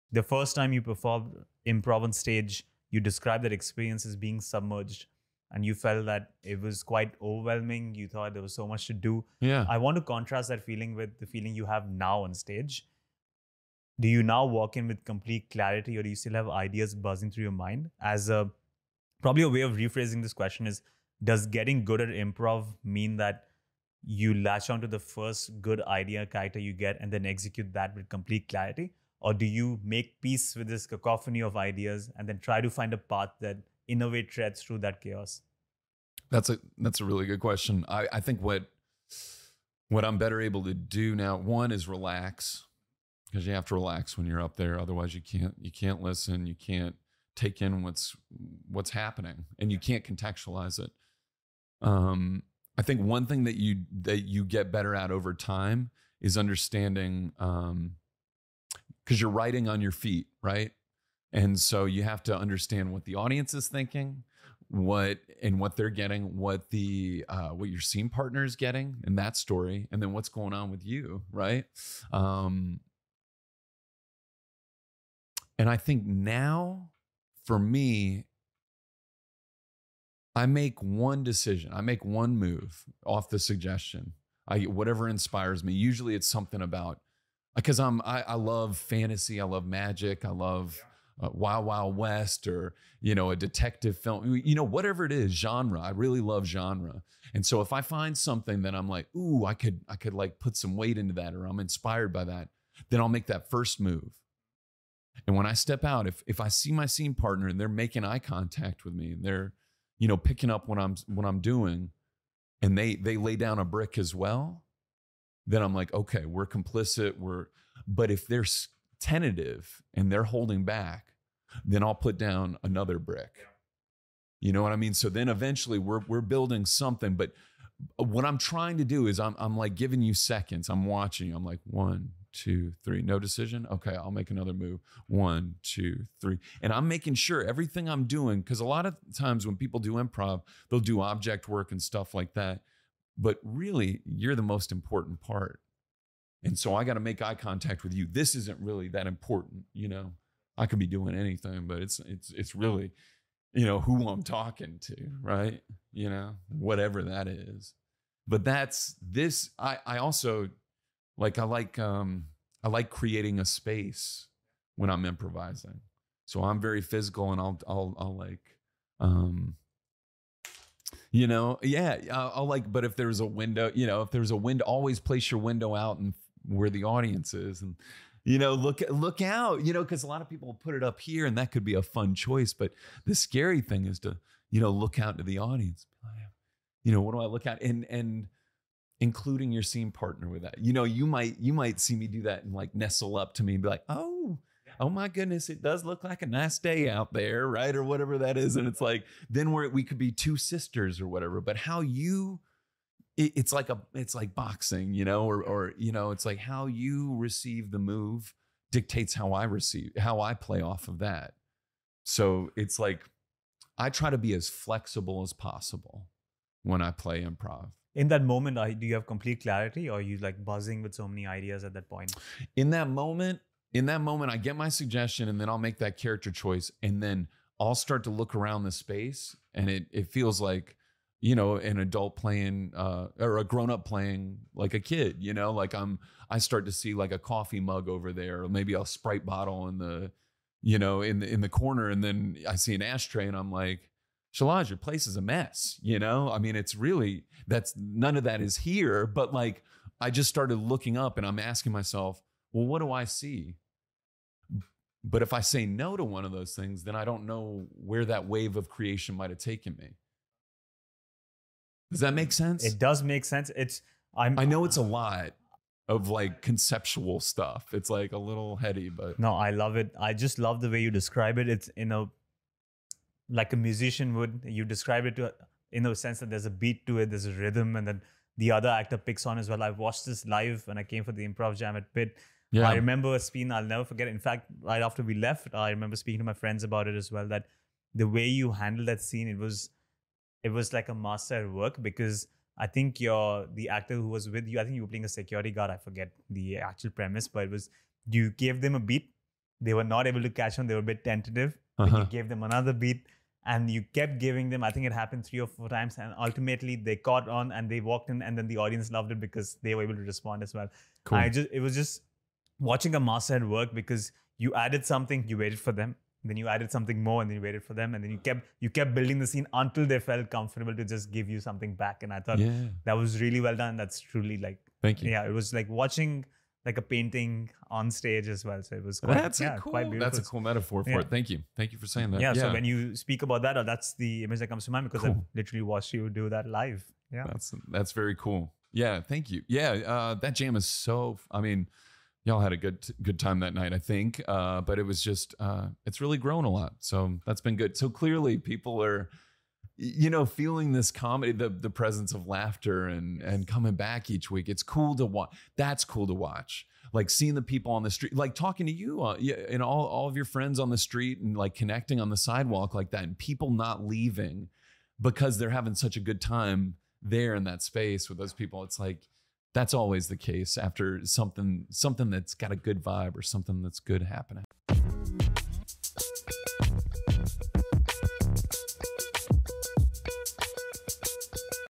How Do You Learn Improv? | Know Time Jiffies
People's Improv Theatre NYC instructor Patrick Darwin Williams talks about performing improv on stage.